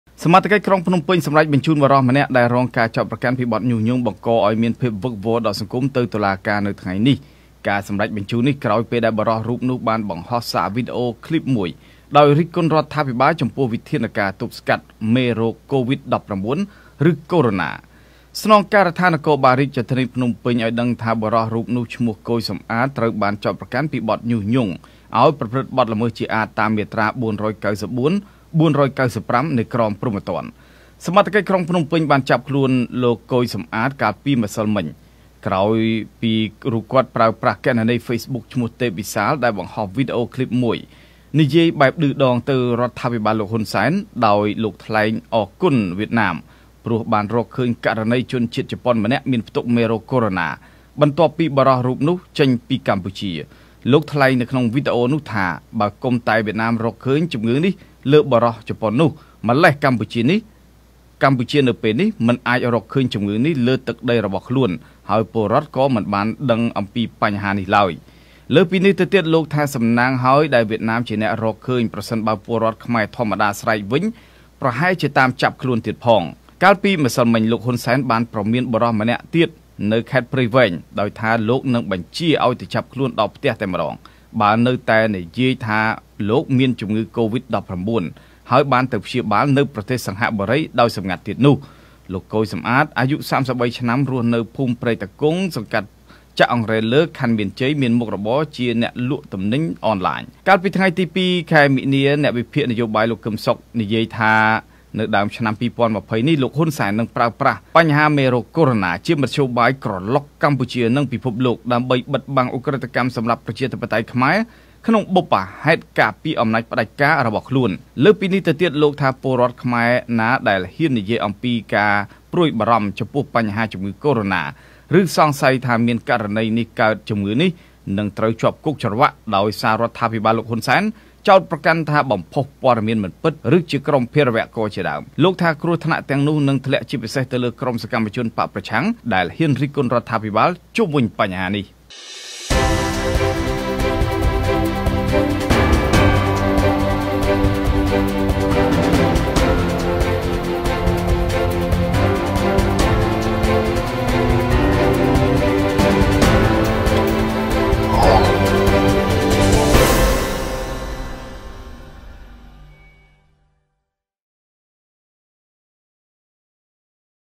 សមត្ថកិច្ចបញ្ជូនបុរសម្នាក់ទៅតុលាការក្រោយរិះគន់រដ្ឋាភិបាលលើបញ្ហាជំងឺកូរ៉ូណា
សមត្ថកិច្ចក្រុងភ្នំពេញ សម្រេចបញ្ជូនបុរសម្នាក់ ដែលរងការចោទប្រកាន់ពីបទញុះញង់បង្កឱ្យមានភាពវឹកវរដល់សង្គម ទៅតុលាការនៅថ្ងៃនេះ។ ការសម្រេចបញ្ជូននេះ ក្រោយពេលបុរសរូបនោះ បានបង្ហោះសារវីដេអូខ្លីបមួយ ដោយរិះគន់រដ្ឋាភិបាល ចំពោះវិធានការទប់ស្កាត់មេរោគកូវីត-១៩ ឬ កូរ៉ូណា។